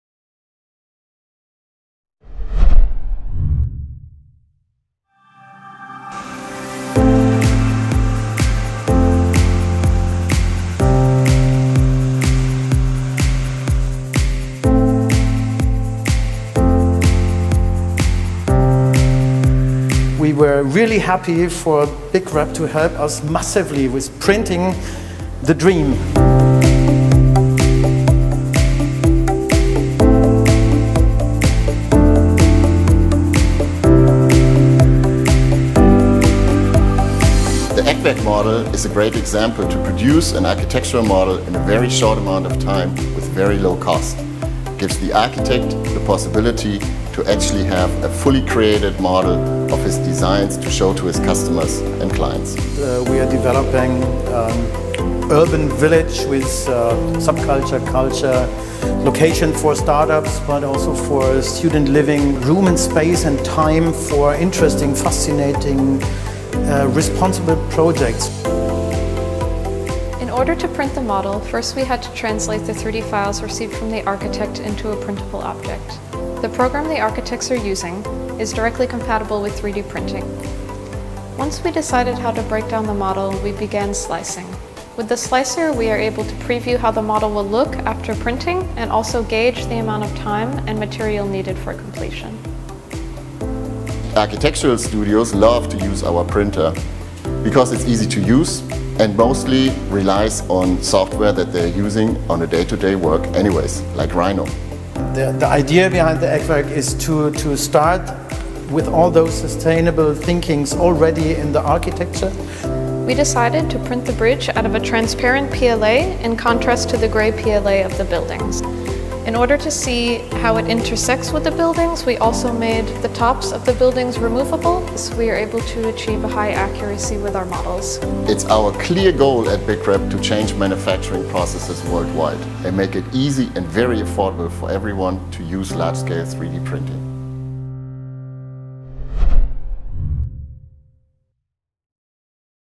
Eckwerk 3D Printing Large Architectural Model BigRep Large-Scale 3D Printer
The Eckwerk architectural model is a complete large scale 3D printed model made on the BigRep One printer.

See how a BigRep industrial 3D printer effortlessly produces a complex architectural model in full detail and with a beautiful, high-impact finish.